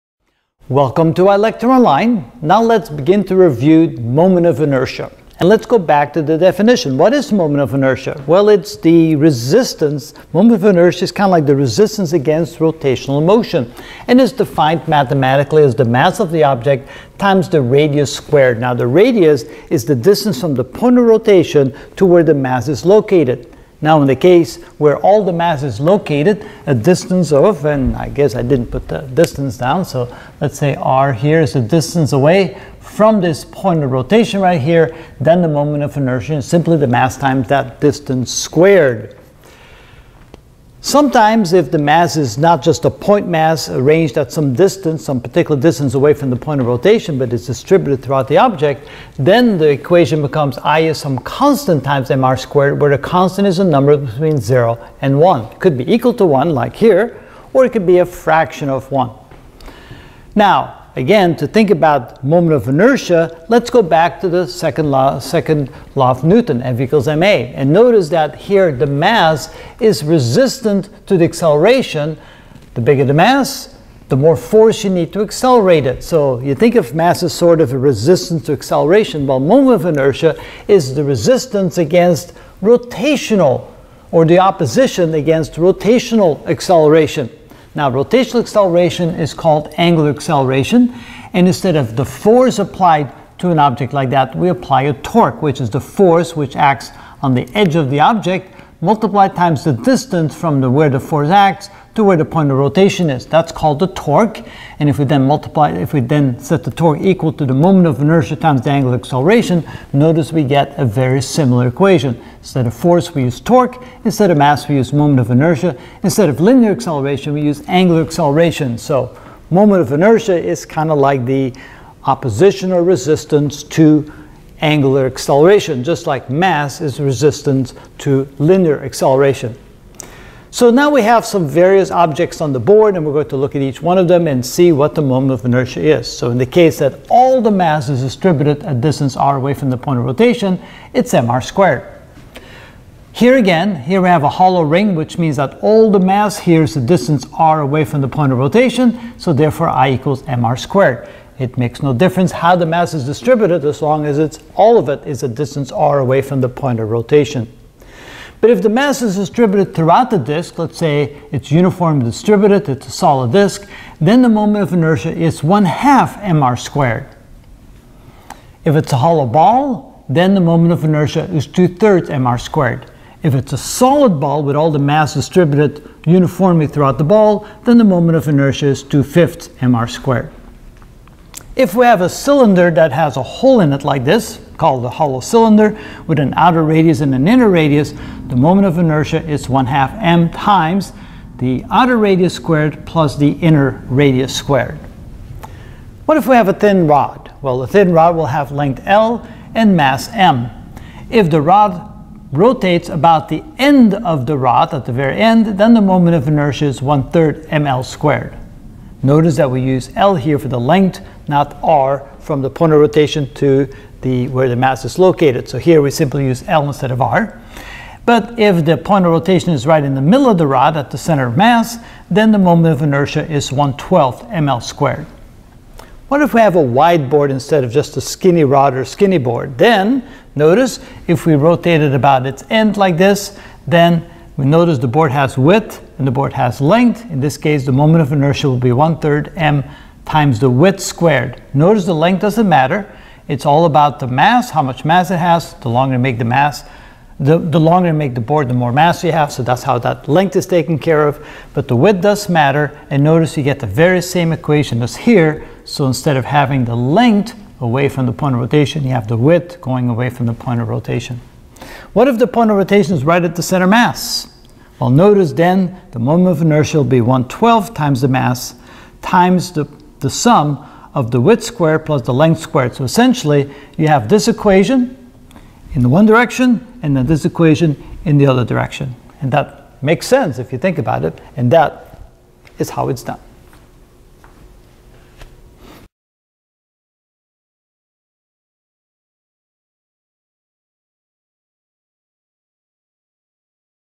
Physics Review: Moment of Inertia Introduction (Part 1)
(Part 1) We will learn how to review and study quickly on the Introduction of Moment of Inertia of a solid mass on a string, hollow ring, solid disk, hollow ball, solid ball, hollow cylinder, thin rod, and wide board.

Title seach on other plateforms:  Physics Review: Rotational Motion (Part 2)

Title search on other plateforms:  Physics Review: Moment of Ineria Introduction (Part 1)